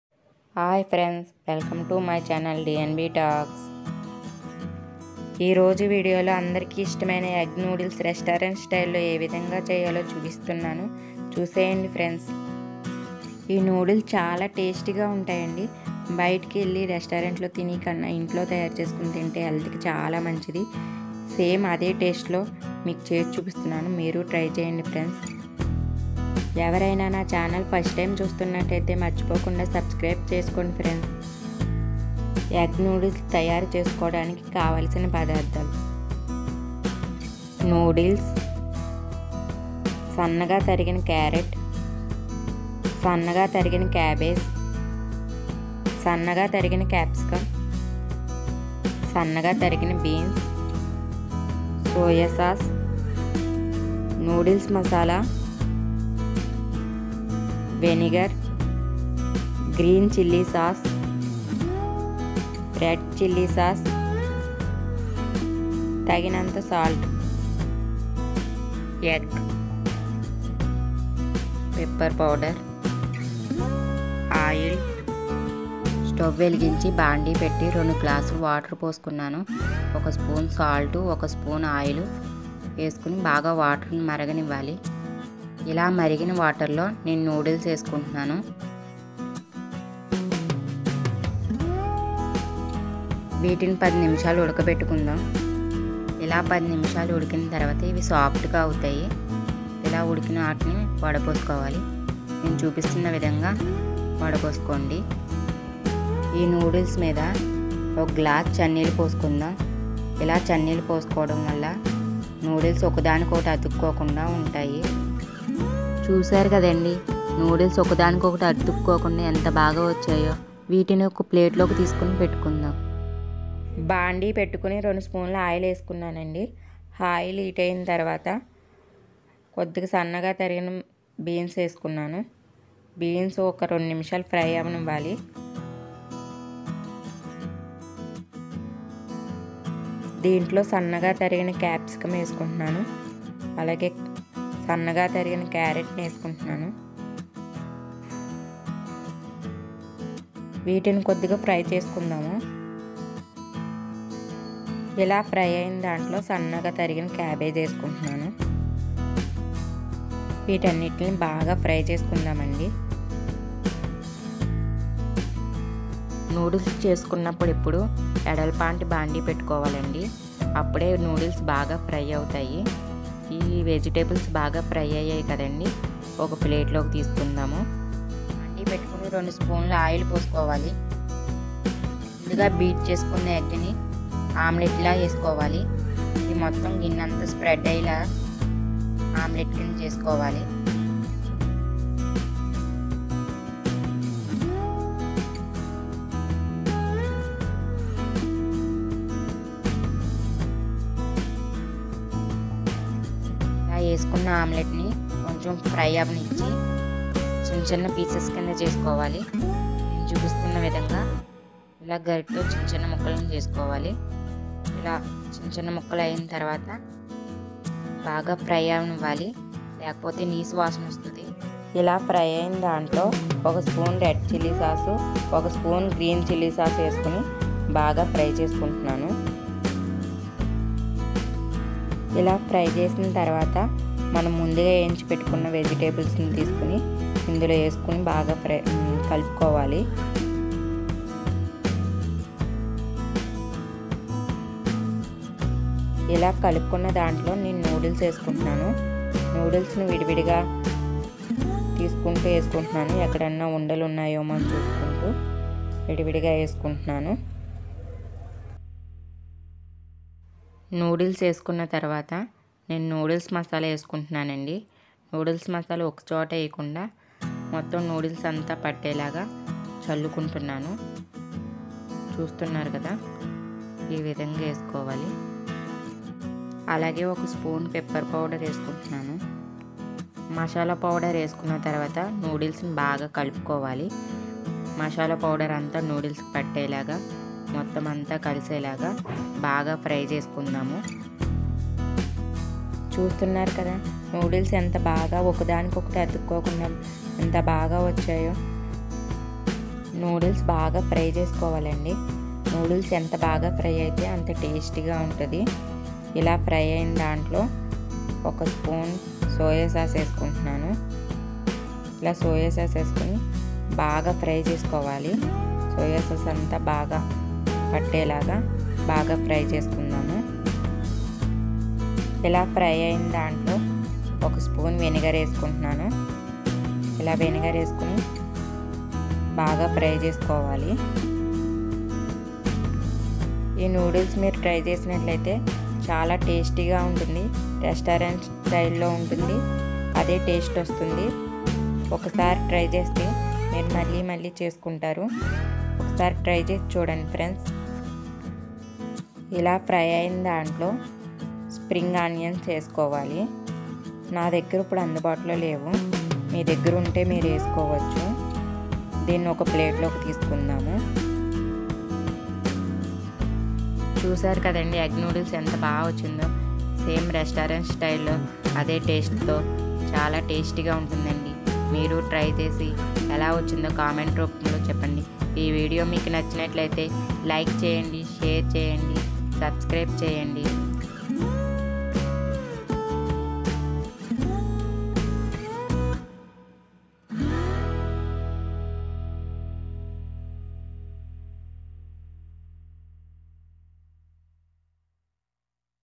Egg Noodles|Restaurant Style in Telugu|ఎగ్ నూడుల్స్ రెస్టురంట్ స్టైల్ ఇంట్లోనే చేయటం చుడండి
Please watch how to make perfect egg noodles restaurant style at home video in Telugu.